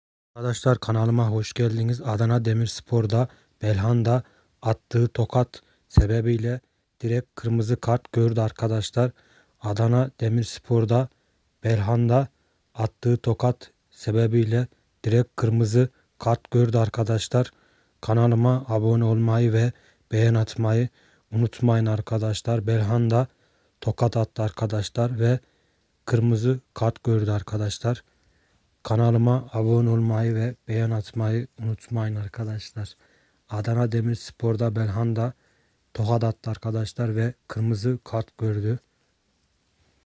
Belhanda Bruno Peres'e Attığı Tokat Gerekçesiyle Kırmızı Kart Gördü
Arkadaşlar kanalıma hoş geldiniz❤